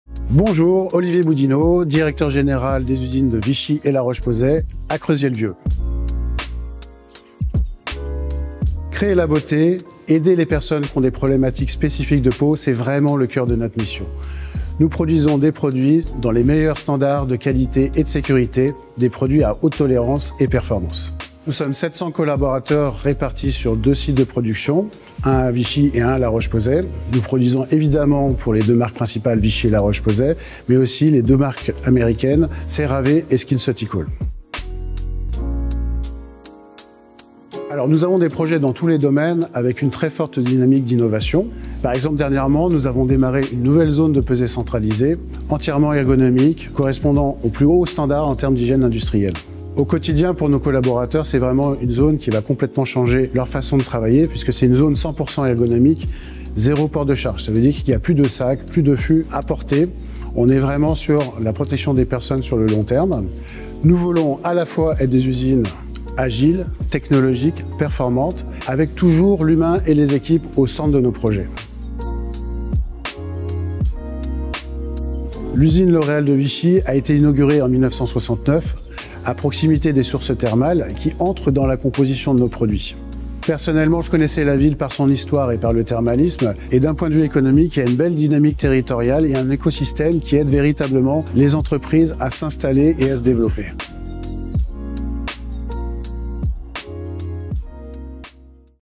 Interview vidéo : Olivier Boudinot, directeur général des usines Vichy et la Roche Posay
L'usine Cosmétique Active Production basée à Creuzier-le-Vieux  fabrique des produits cosmétiques, notamment de la marque Vichy, pour la division Cosmétique Active de L’Oréal, avec une spécialisation dans le domaine du soin de la peau à base d’eau thermale.

Olivier Boudinot, directeur général des usines Vichy et la Roche Posay, nous présente les projets et la dynamique d'innovation.